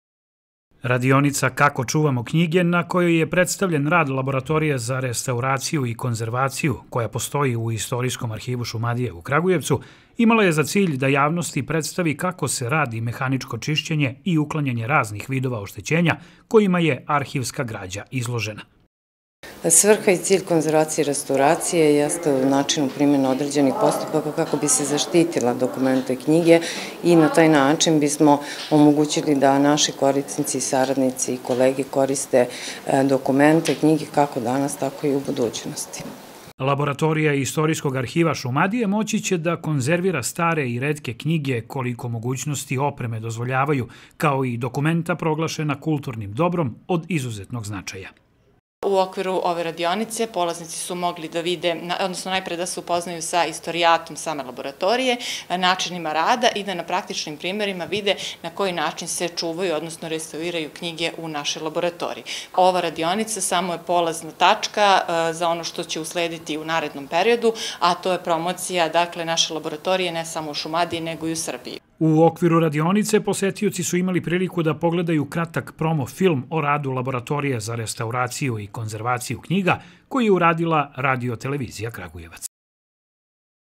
Kako čuvamo knjige?
U Istorijskom arhivu Šumadije predstavljen je rad Laboratorije za restauraciju i konzervaciju knjiga.